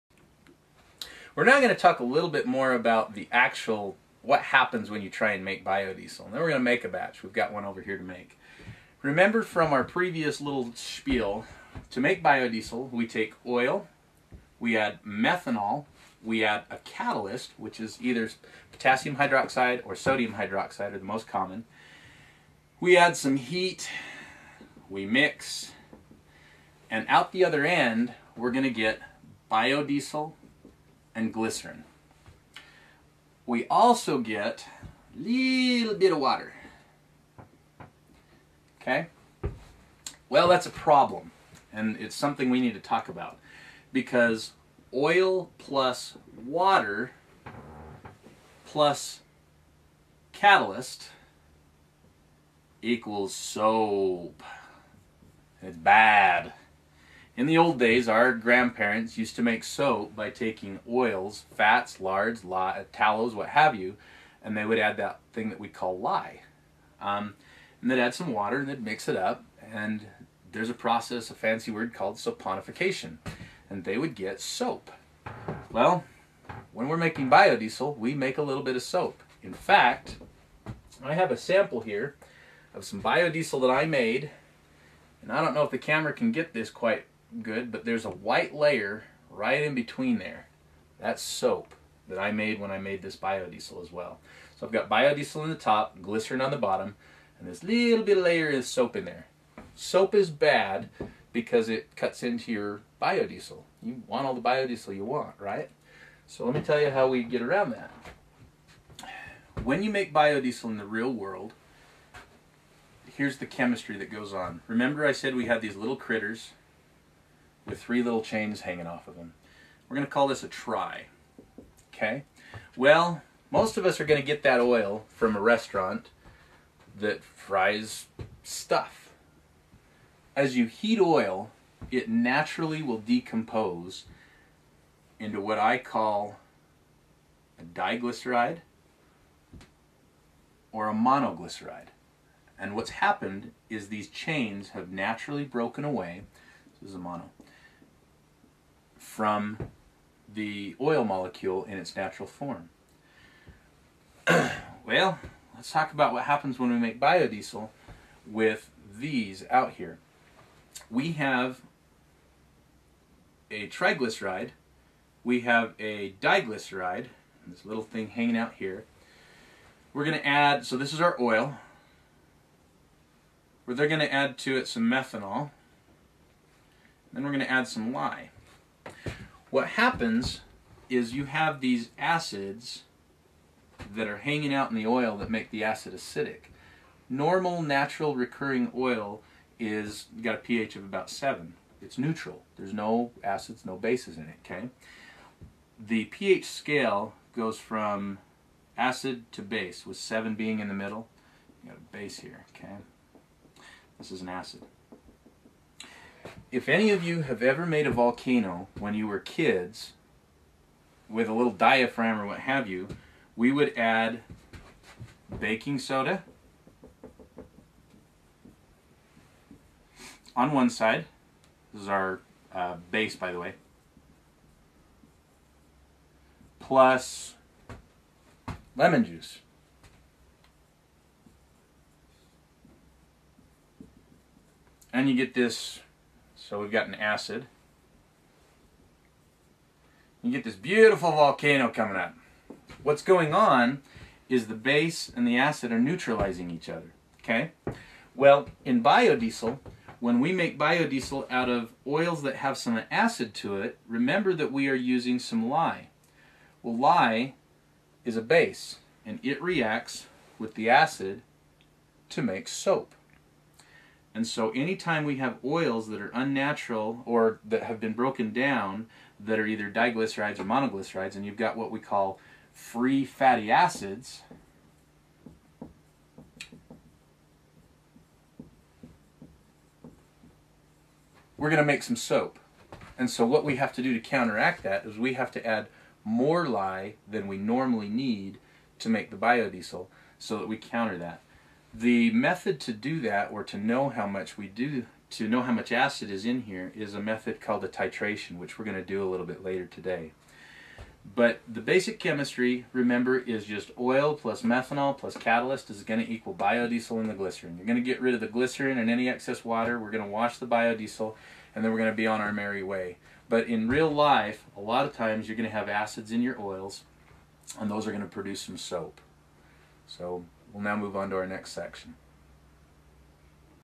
The Chemistry of Biodiesel (Part 2)
The Guys at Parley's Diesel Performance teamed up with Graydon Blair of Utah Biodiesel Supply to put a lot of common biodiesel and diesel performance questions to rest and show how to brew biodiesel and answer the question of what happens when you brew biodeisel that makes it able to run in any diesel engine.  Thanks again Graydon!

Visit Parley's Diesel Performance for all your diesel performance part needs.